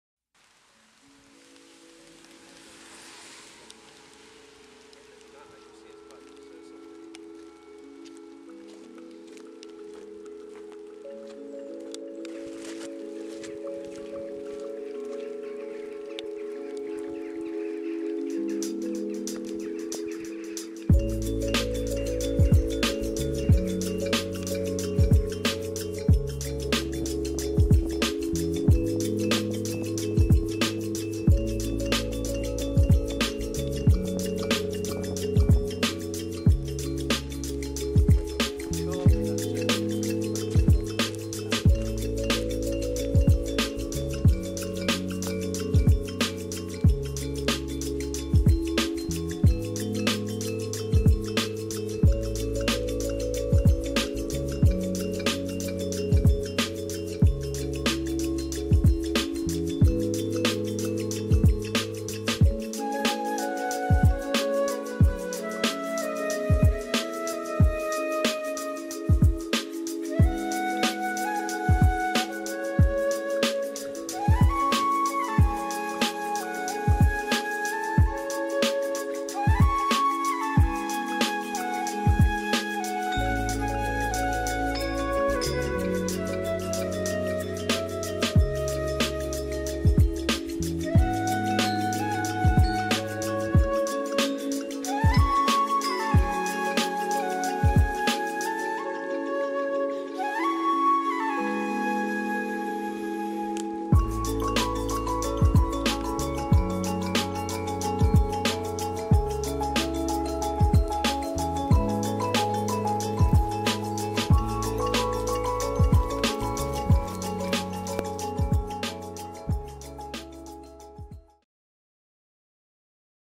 Walk With Me
A chilled out walk with Thad and Dad